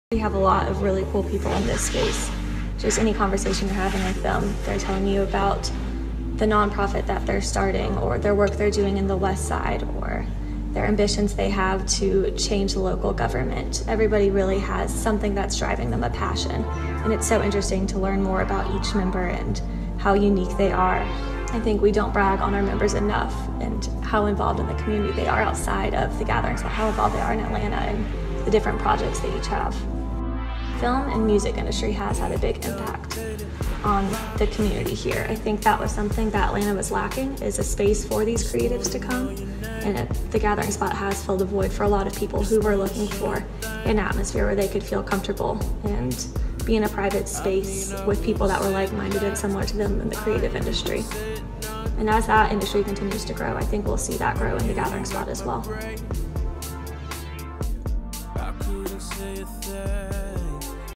TGS IN THE LAB Episode #2: MEMBERSHIP
IN THE LAB is a short form video series that spotlights The Gathering Spot’s unique team, culture and point of view.

In Episode 02, Membership Director, Regan Bercher shares a snapshot of our diverse membership community.